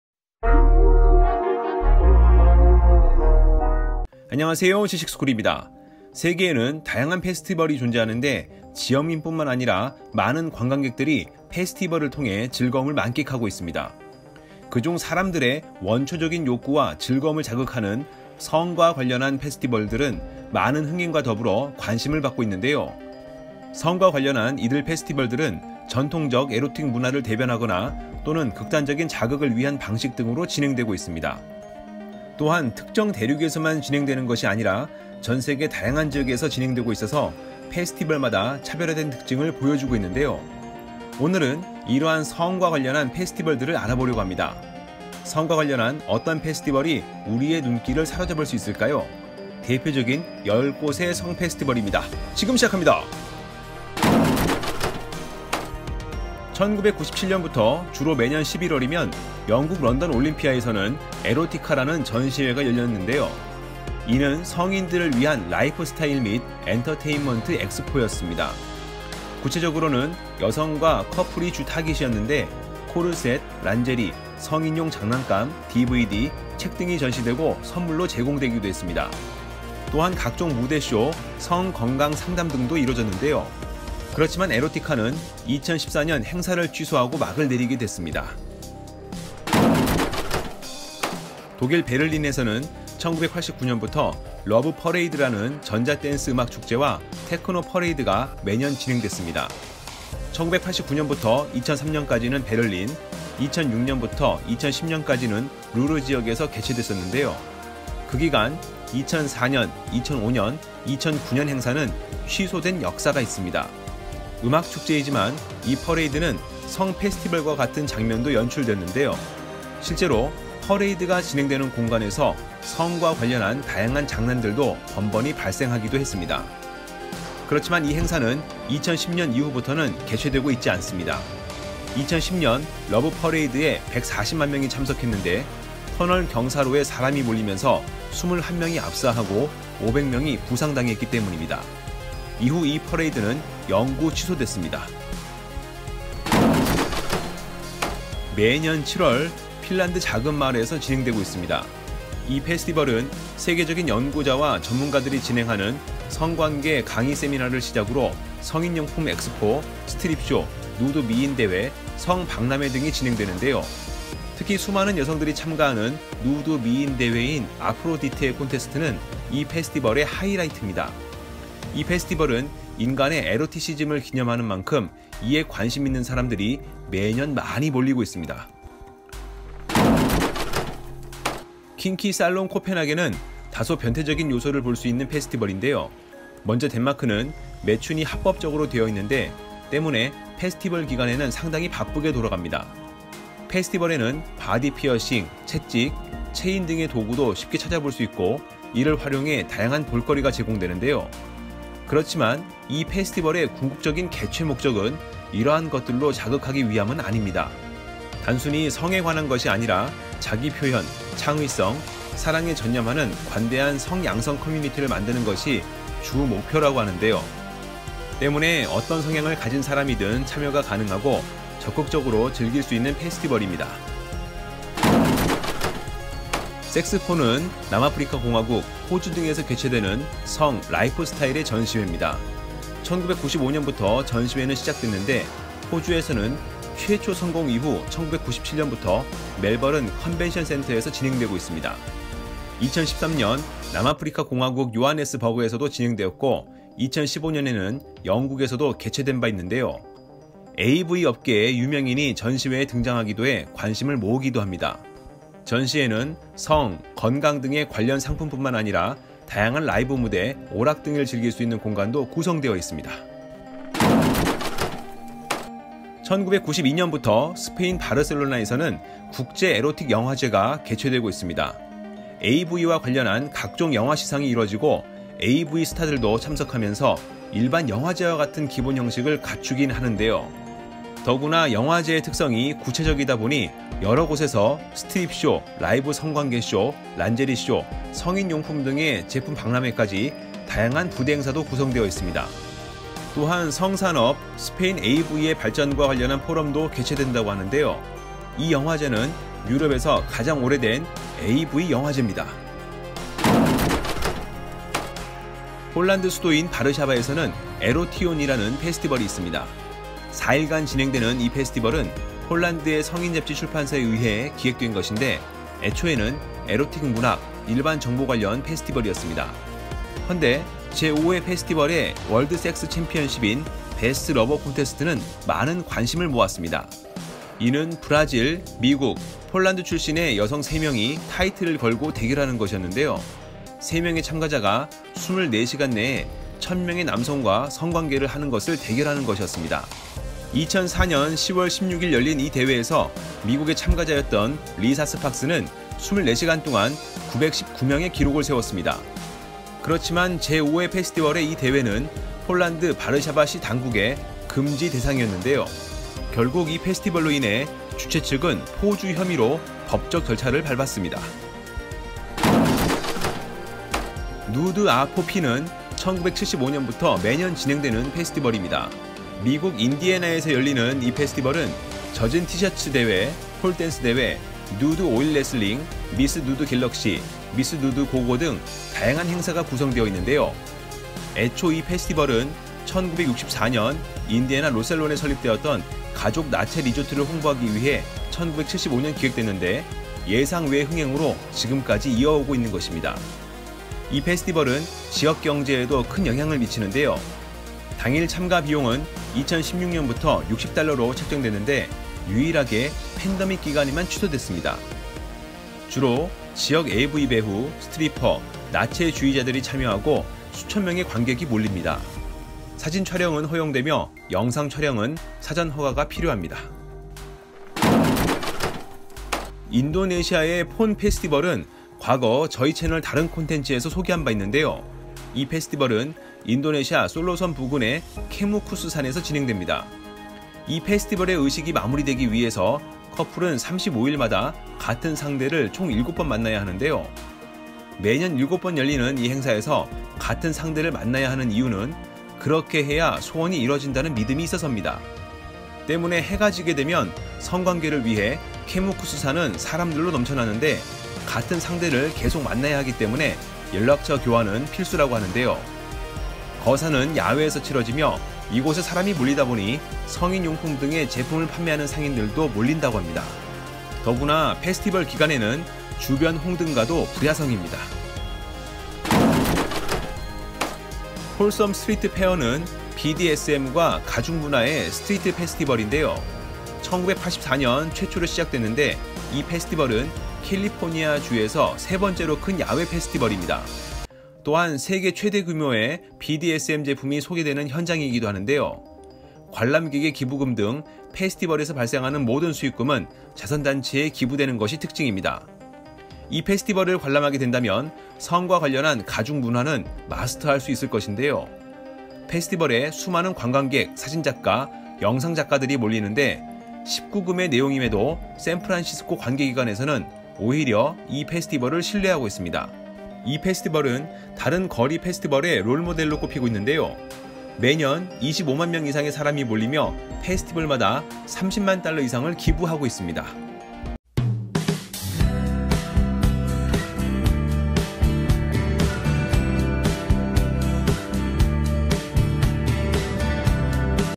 우리나라에 없는 대표적인 19+ 성인 페스티벌 Top 10
사람들의 원초적인 욕구와 즐거움을 자극하는 성과 관련한
페스티벌들은 많은 흥행과 더불어 관심을 받고 있다.
성과 관련한 어떤 페스티벌이 우리의 눈길을 사로잡을 수 있을까?

* 자료출처
Quinn Dombrowski - The apple - Folsom Street Fair
Steven Damron - Dog sled - Folsom Street Fair 2012
Jonathan Chen - Folsom Street Fair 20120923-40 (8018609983)
Tom Hilton - Folsom Street Fair San Francisco 2017 20
Tim Bartel - In front of the stage at Folsom Street Fair
Midori - Odalan procession
Naked Bare Nude - Nude girls
NAP310 - Nudes-A-Poppin' 2009-128
Naked Bare Nude - Nudes-A-Poppin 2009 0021
CharleswclarkII - Nudes a Poppin 2018
Naked Bare Nude - Naturist sunbath
Zboralski - Krzysztof-Garwatowski-2009
Lisasparks.com - Lisa Pic
Wistula - Police vehicles at independance march 2019
Franz Xaver Kuhn - Monique Covet Berlin Erotic Film Festival 2004
Daniel Gómez - FICEB 2006
matadorstuff - Sharka Blue crop
Martinasanz - Roberto Chivas
Mpellerano - Kinky Trans
Eva Rinaldi - Sexpo Models
Eva Rinaldi - Sexpo-3
Eva Rinaldi - Sydney Sexpo 2013
Sexkino, Odense Denmark - Sexkino Odense
Diogenes64 - Blonde woman wears a ball gag
JIP - Bodypainted Finnish couple
JIP - CycloNudista 2018
Naked Bare Nude - Body painting 16
JIP - WBF female 2
Arne Müseler - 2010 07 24 loveparade 2010 duisburg 0141 cropped
Arne Müseler - Loveparade 2010 duisburg tunnel ramp
Pradeur - Nude girl hands out flyer at Loveparade 04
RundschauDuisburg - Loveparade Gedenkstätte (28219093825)
JD Lasica - Loveparade (46338372)
London Pixels - Exhibitor2008
Brian Minkoff, London Pixels - Powertool
Steven Damron - Blue horse - Folsom Street Fair 2012
Tom Hilton - Folsom Street Fair San Francisco 2017 12
Quinn Dombrowski - Flogging tips
Quinn Dombrowski - Jewel-studded leopard man
istolethetv - 547W27 - Folsom Street Fair